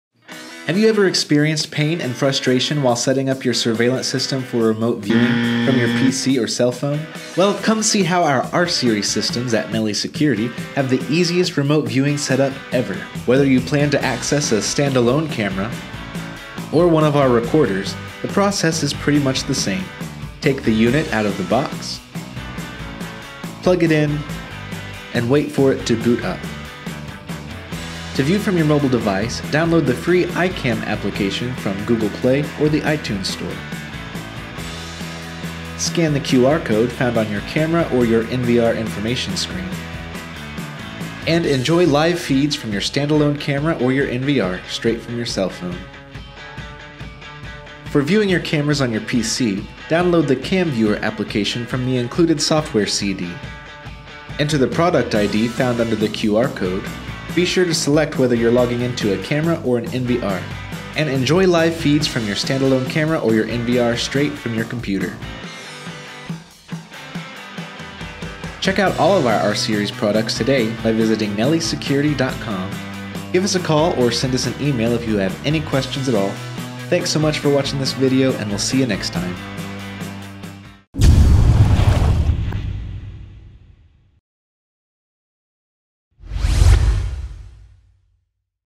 Remote Viewing Has Never Been So Simple! View Surveillance Feeds Straight From Your Phone & Computer
Have you ever experienced the pain and frustration of trying to get your surveillance system set up for remote viewing? Then you'll love how easy our new R-Cameras are to access from your phone or computer.

Getting set up for remote access with our R-Series cameras and recorders is as simple as scanning a QR code. Access the camera feeds from your phone or your computer. Not tech savvy? No problem! If you know how to open an app on your phone, you can access your cameras anytime and anywhere.